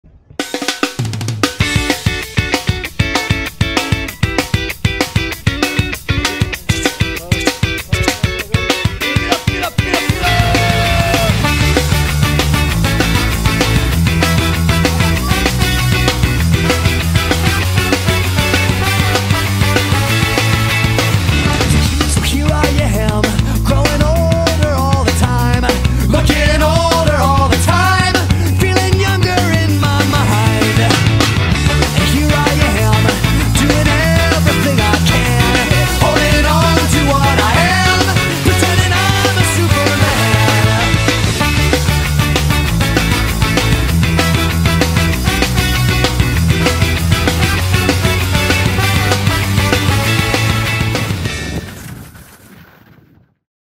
Snow Summit Resort Big Bear CA 2022 season
Taking the boys out for a ride!